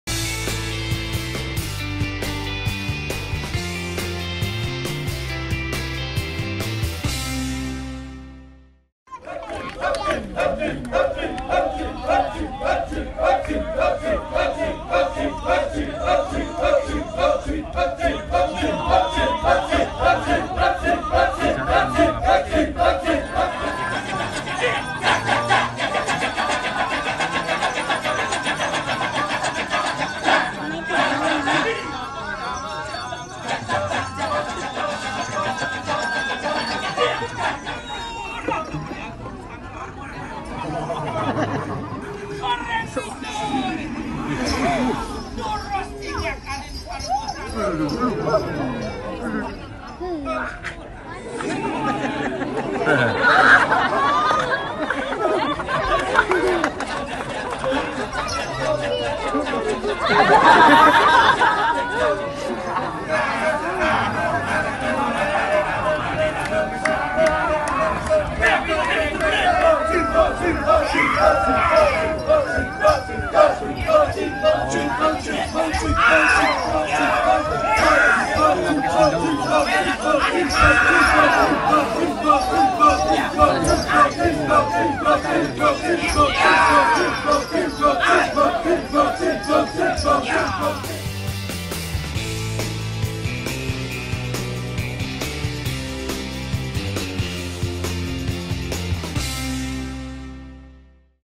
Pertunjukan Kecak & Fire Dance di Pecatu Bali
Assalamualaikum video ini merupakan pertunjukan ciri khas tarian di Bali, merupakan drama Rama & Shinta.
Mohon maaf jika ada kekurangan / salah.